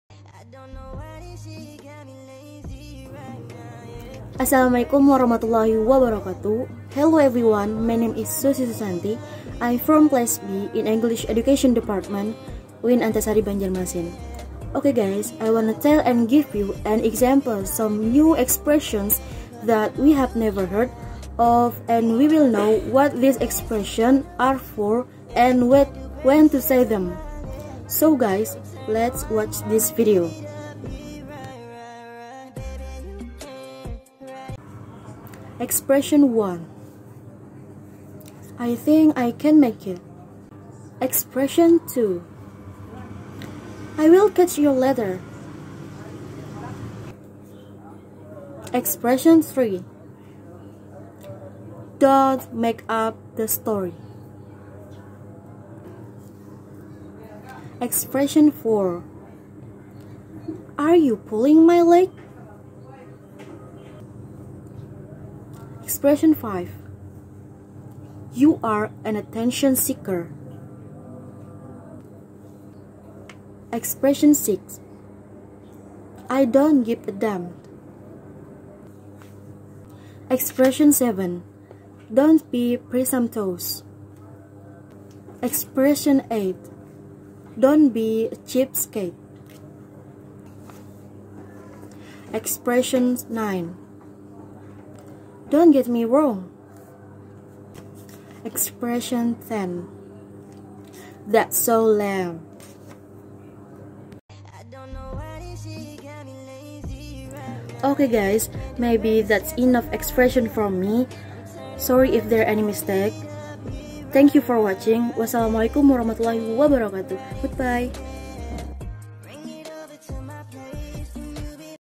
Susi Susanti// Speaking II// Assigment
Bismillahirrahmanirrahim

Hi teman-teman semuanya

Name: Susi Susanti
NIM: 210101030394
Class: TBI B 202
Subject: Speaking II
lecture: Miss Thaibatun Nisa, M.Pd

This video is a Speaking II assignment, there are 10 new expressions. This is part 2, there will be a part 3 later. Don't miss the next video 😂🙏🏻